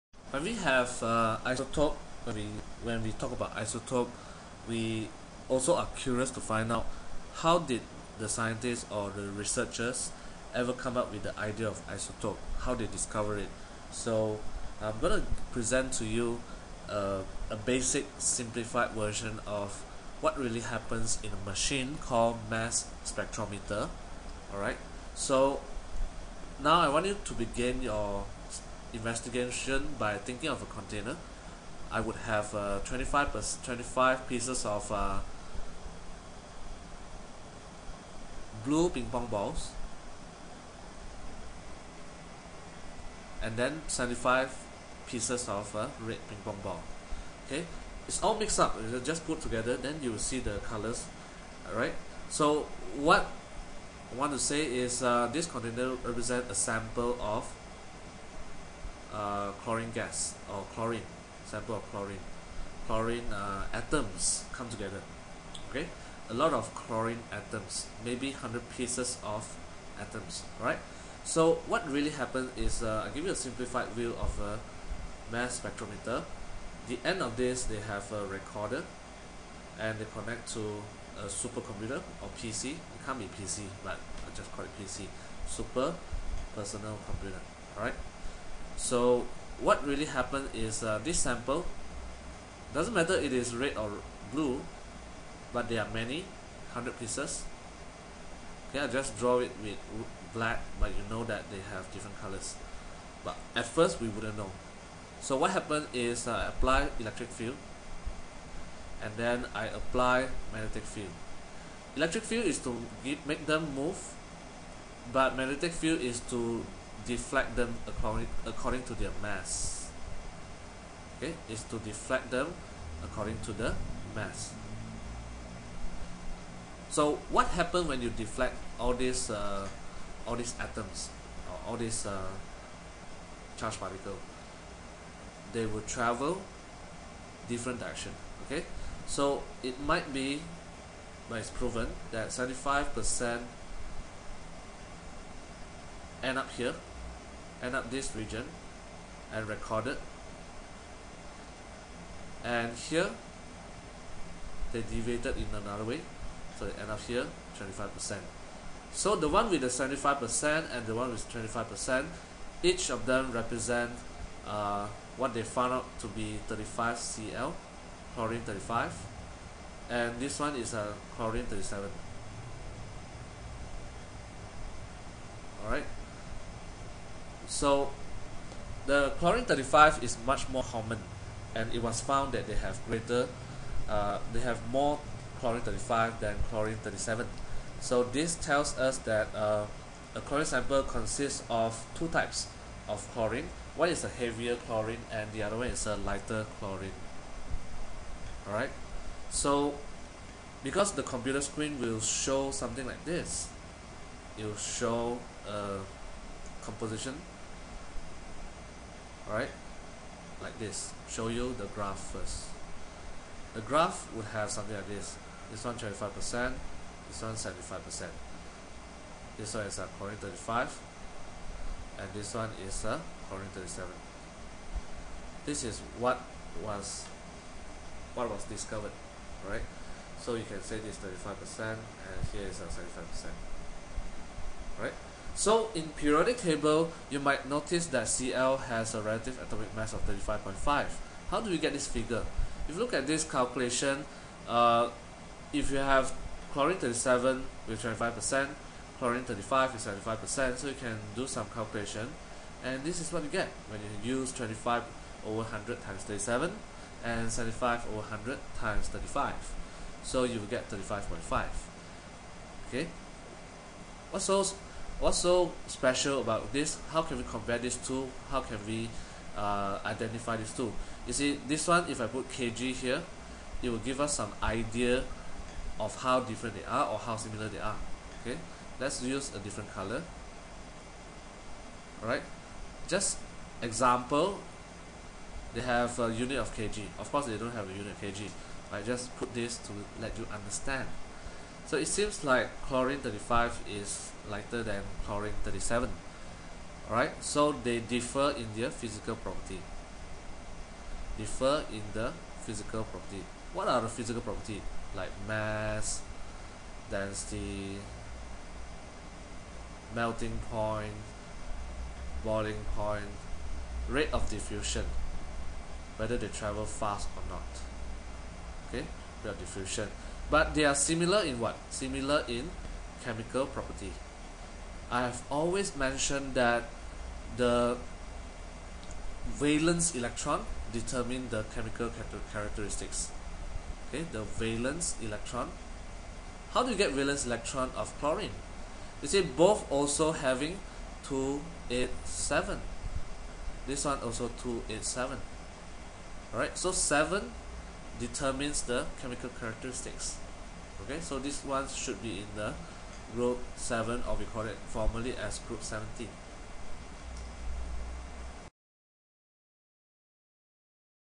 SPM Chemistry How to Understand Isotope
Mass Spectrometer is studied extensively in A Level Physics & Chemistry.
Here is just a simple explanation of what happen if a sample of element is put inside the machine.
The result is isotopes of chlorine, will be separated due to their difference in their masses.